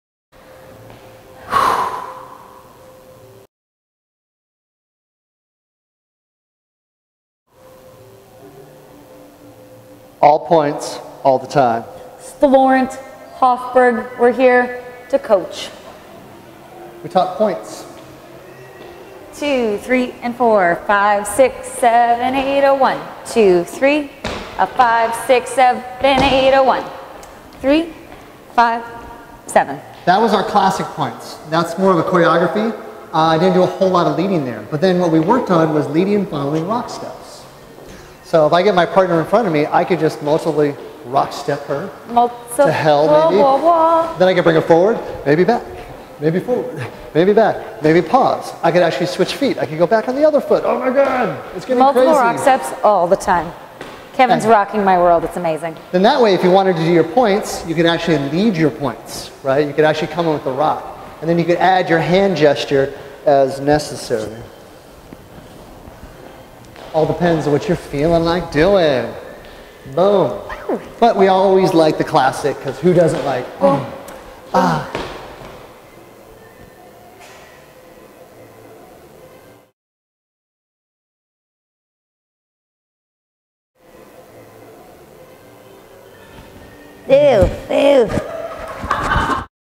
February 2013 — Level 3 Lindy — Week 3
Kevin and Jo review the moves and exercises from this week's Level 3 class: Fancy Moves.

Instructors: Jo Hoffberg and Kevin St. Laurent